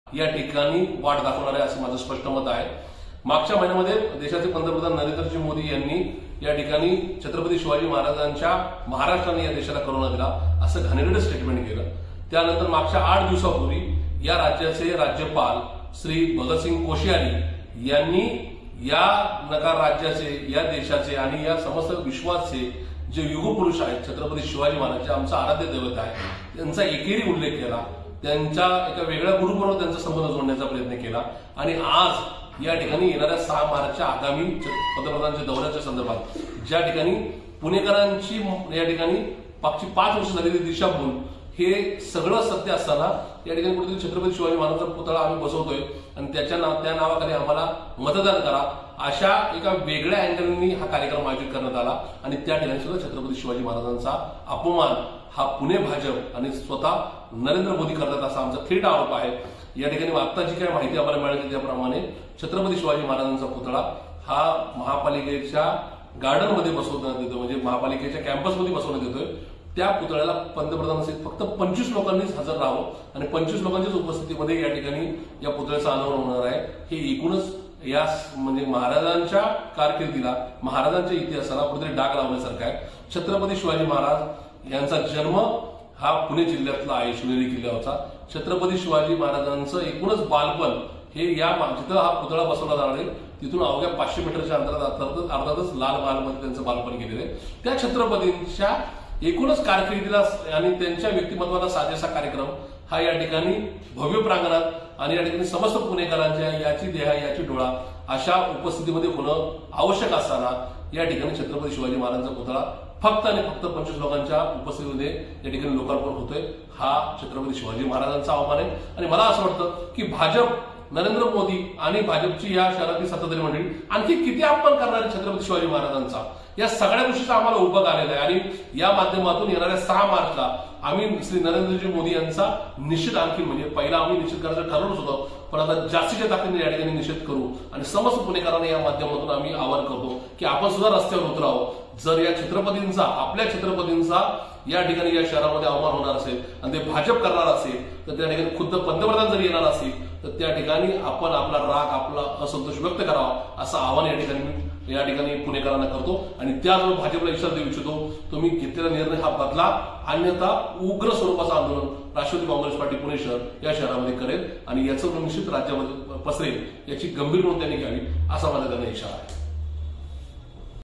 PUNE | छत्रपती शिवाजी महाराज यांच्या पुतळ्याचा लोकार्पण सोहळा पुणेकरांच्या उपस्थितीत होणं आवश्यक
पंतप्रधान नरेंद्र मोदी यांच्या हस्ते महापालिकेच्या आवारात उभारण्यात येणाऱ्या छत्रपती शिवाजी महाराज यांच्या पुतळ्याचे अनावरण फक्त 25 लोकांच्या उपस्थितीत करणे हा महाराजांच्या अपमान
मात्र, भाजप केवळ 25 लोकांच्या उपस्थितीत लोकार्पण सोहळा आयोजित करुन महाराजांचा अपमान करत आहे
सहा मार्चला राष्ट्रवादी पंतप्रधान नरेंद मोदी यांना रस्त्यावर उतरून निषेध करणार
भाजपचने घेतला निर्णय बदलला नाही तर राष्ट्रवादी उग्र स्वरूपाचे आंदोलन करेल

BYTE - प्रशांत जगताप, शहराध्यक्ष राष्ट्रवादी काँग्रेस